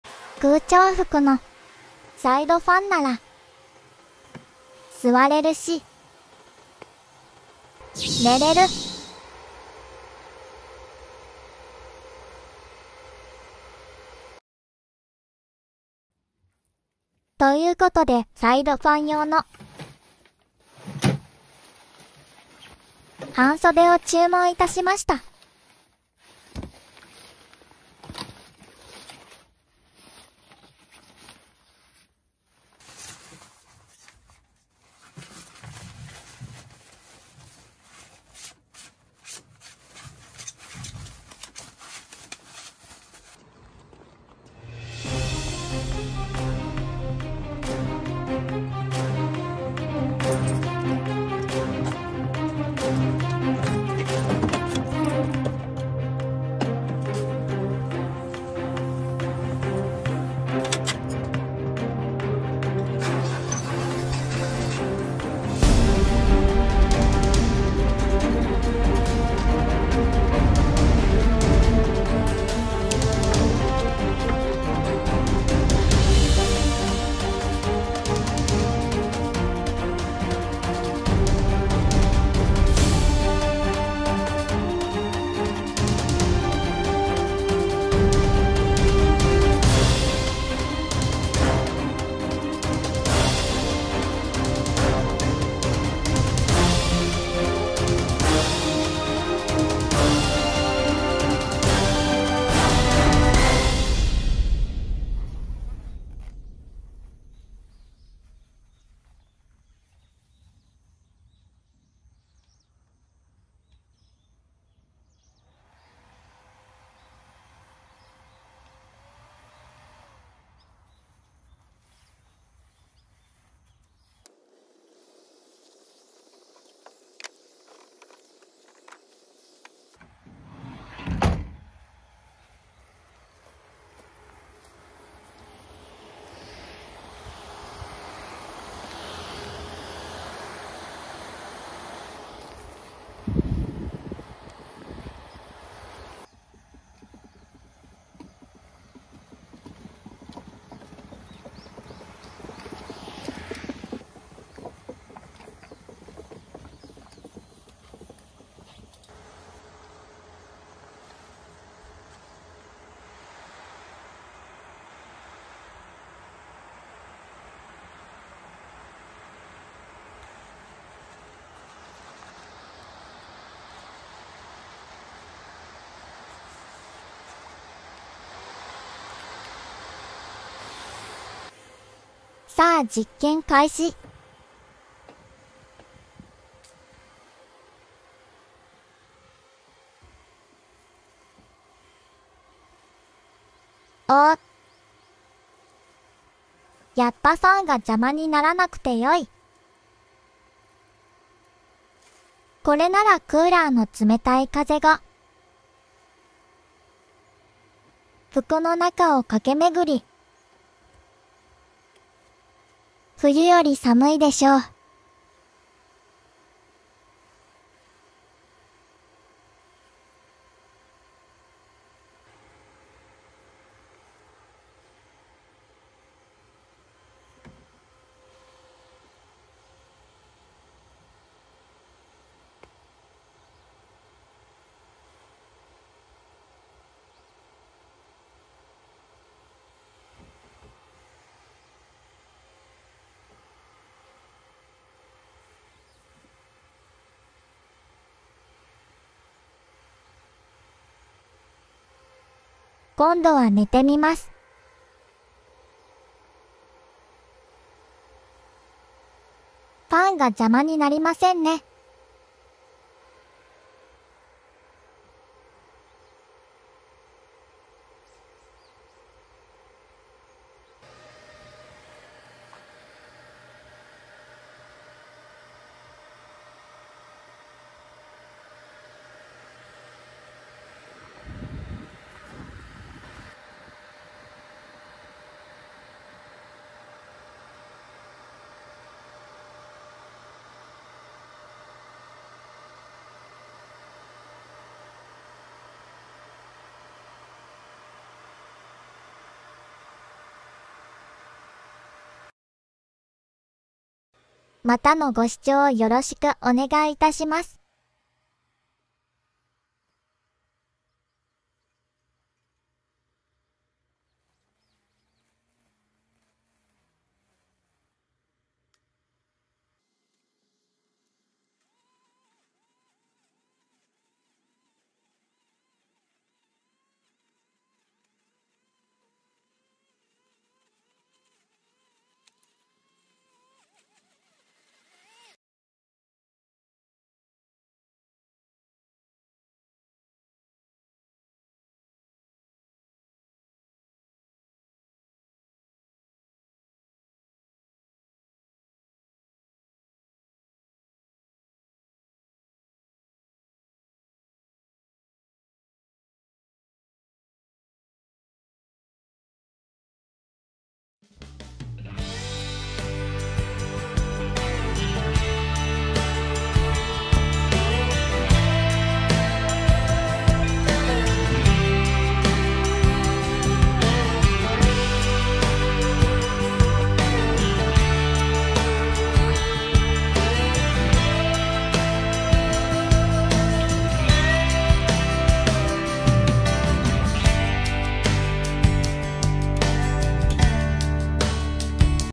空調服のサイドファンなら車の中でも転べる！ 服を脱がなくても運転が出来る 車便利グッズ
音楽素材→YouTubeオーディオライブラリー

オシムラ( oshimu24 )のオリジナルアイテム通販 ∞ SUZURI（スズリ）

車便利グッズ　空調服サイドファンでした^_^